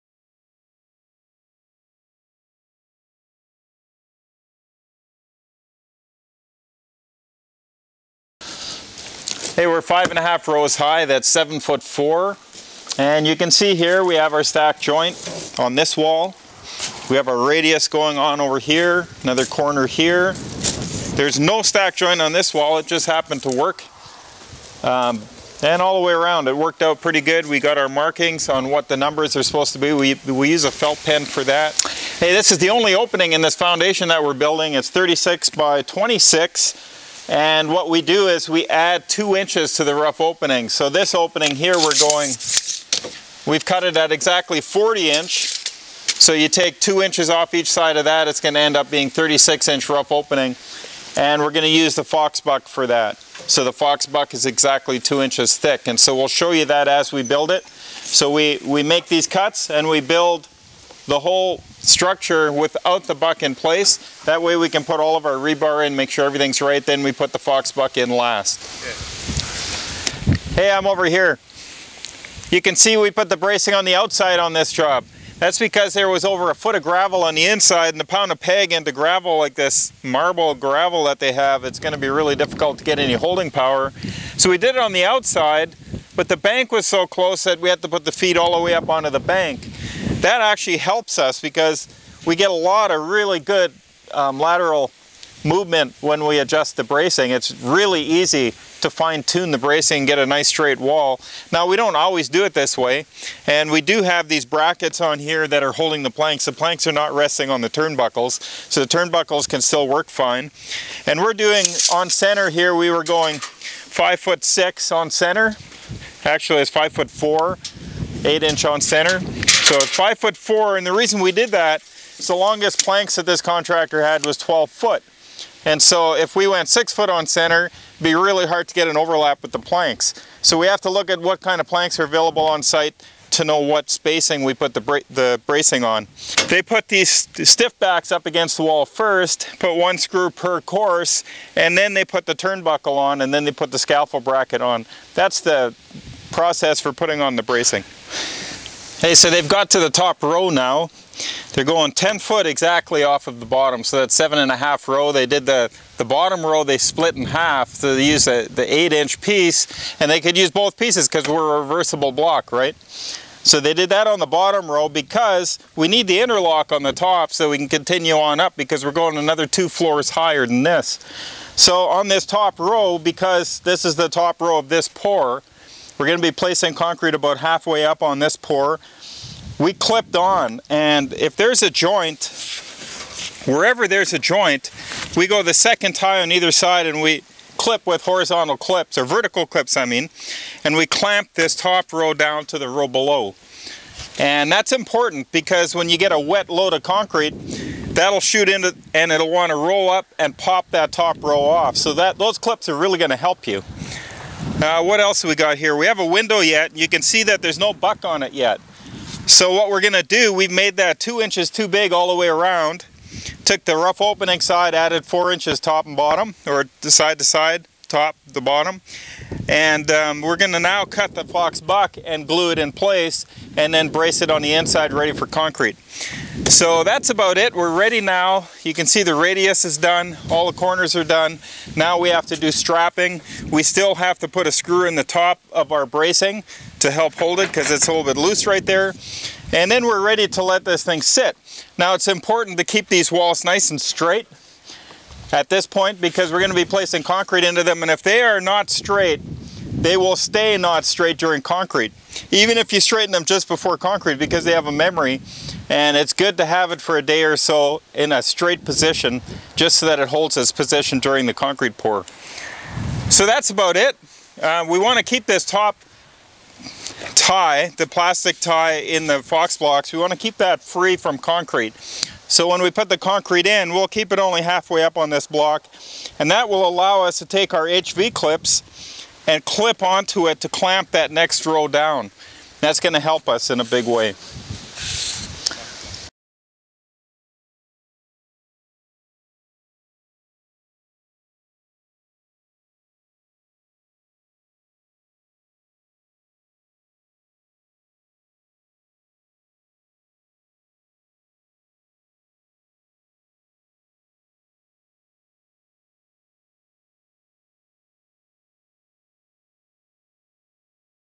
Building your ICF home - Completing Block Placement
Third video of a complete Insulated Concrete Form (ICF) Home build. This series of videos will capture many real life parts of an actual Fox Block ICF home build. We will try to get a new video posted each Week during the build. Thanks to evolvinghomes.ca for allowing us on site.